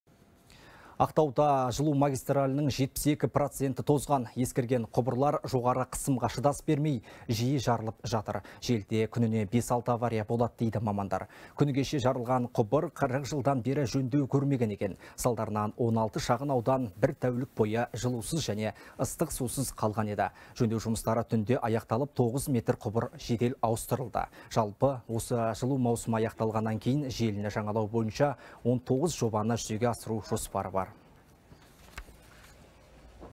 Ақтауда жылу магистралінің 72% тозған
Ақтауда жылу магистралінің 72 проценті тозған.